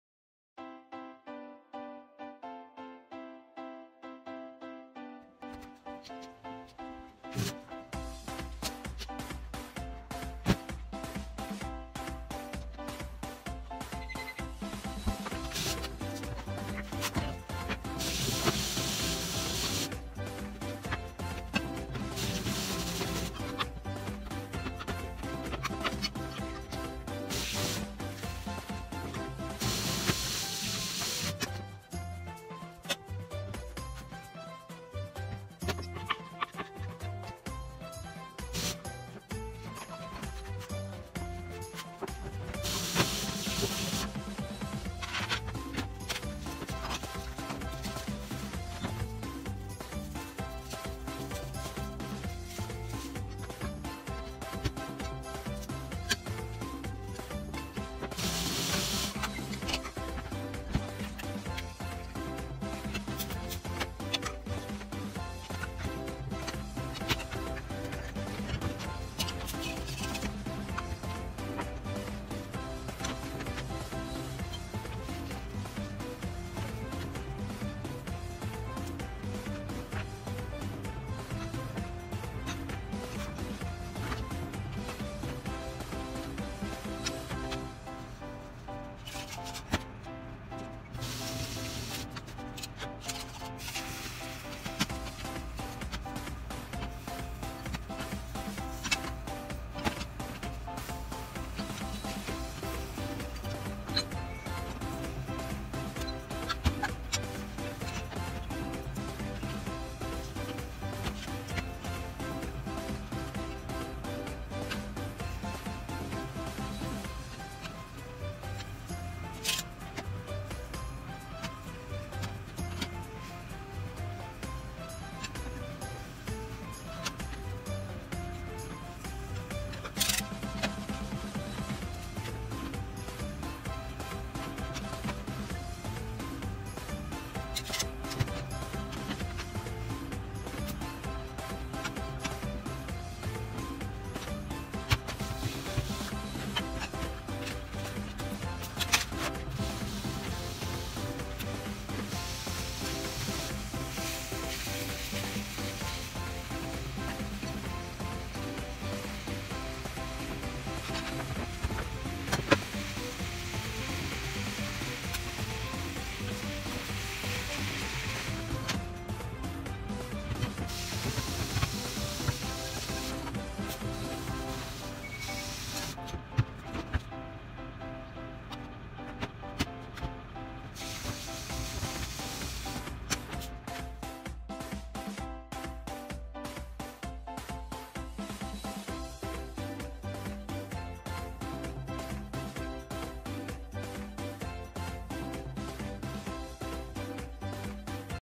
2018/02/03 夕飯【手巻き寿司でミニ恵方巻きをつくったよ】
ただ料理をしてるだけの動画です💦今日は節分で恵方巻きの予定だったけど子供たちのリクエストで手巻き寿司でミニ恵方巻きにしました😊
撮影から編集までiPhoneだけでやってるので画質が悪いのはご了承下さい🙇‍♀️
クックパッドではchiko✻の名前でレシピupしてます👩🏻‍🍳
愛用中の調理器具などは↓に載せてます🍽